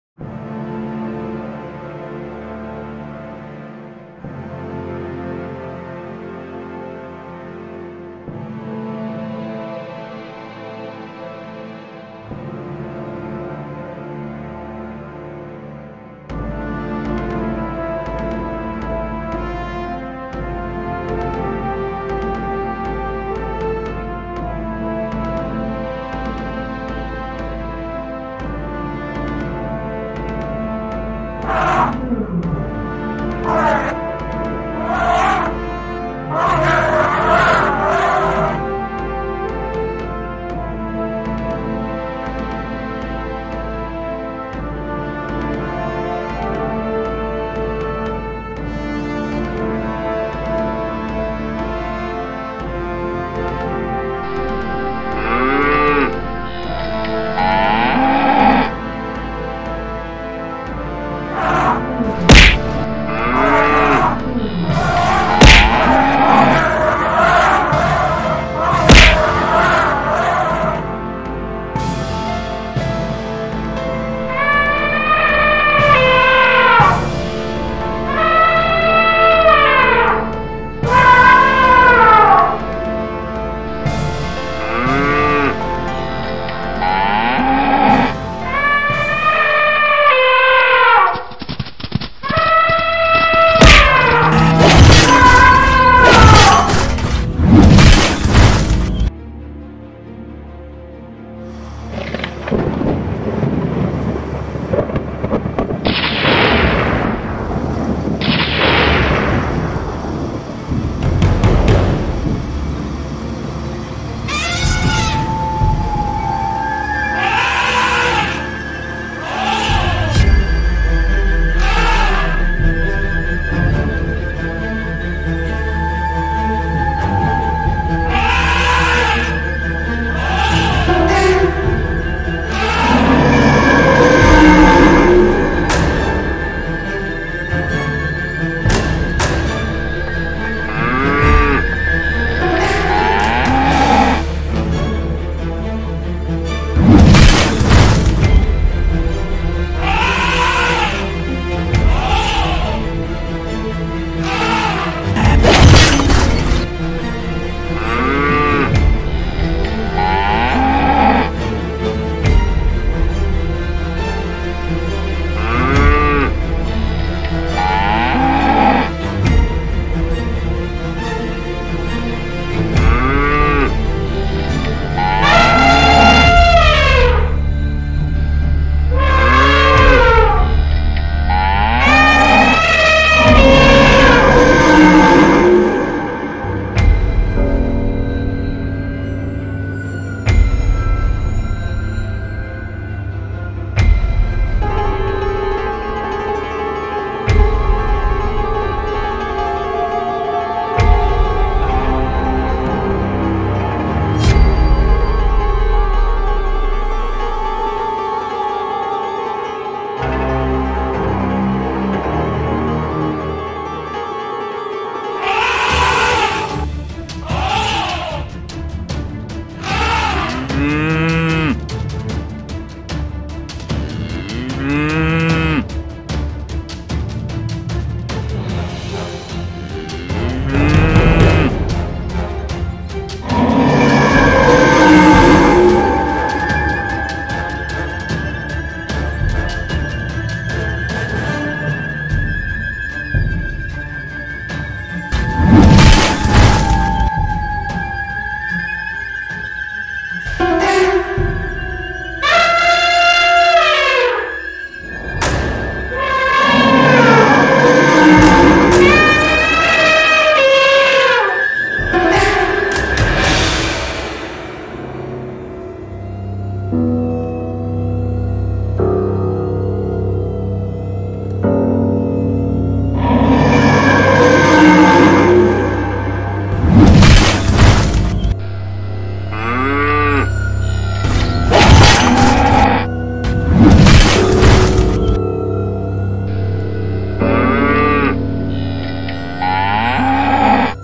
3 Giant Bulls Vs Mammoth Elephant turns into Zombie Mammoth Fight Black Zombie Mammoth Save 3 Bulls
3 Giant Bulls Vs Mammoth Elephant turns into Zombie Mammoth Fight Black Zombie Mammoth Save 3 Bulls Animal Epic Battle